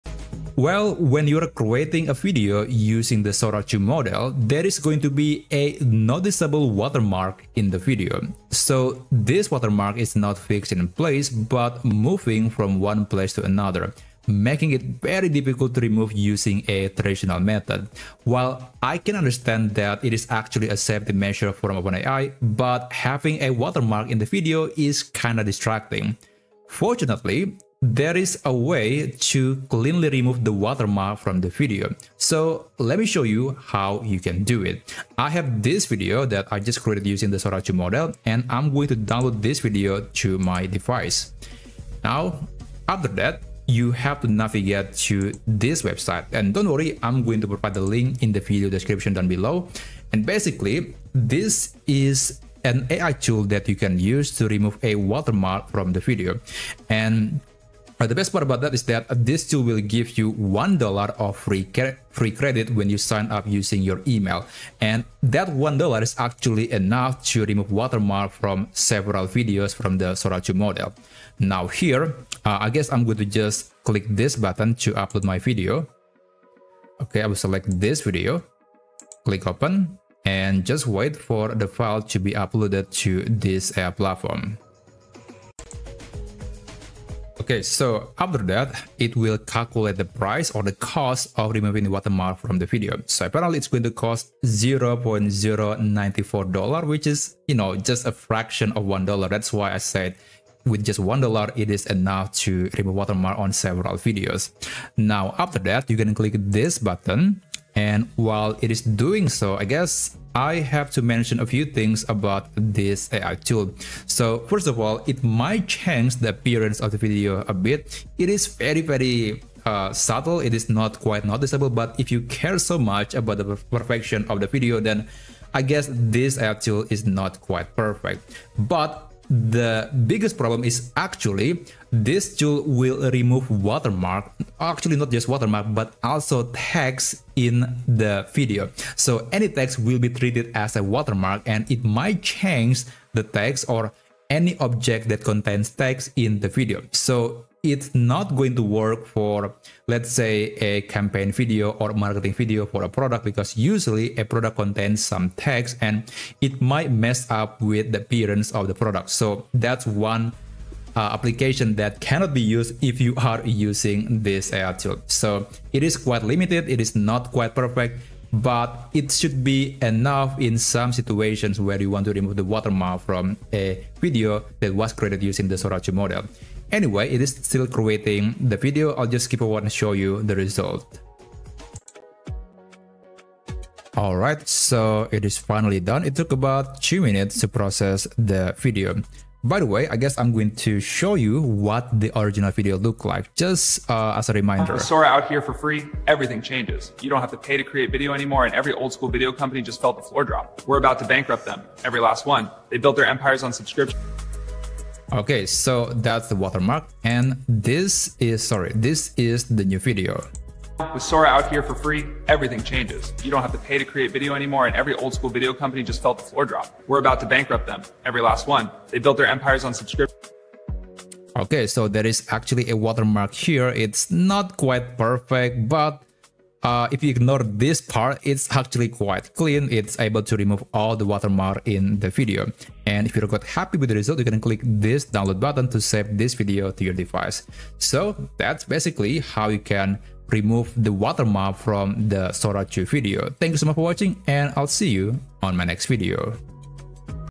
How to INSTANTLY Remove Moving Watermark from Sora 2 Videos
👉  In this video, I will show you how to remove watermarks from Sora 2 videos using WaveSpeed AI, a completely free tool that gets the job done without leaving any traces. If you've been struggling with OpenAI Sora watermarks and want clean, professional-looking AI-generated videos, this method will help you achieve that seamlessly.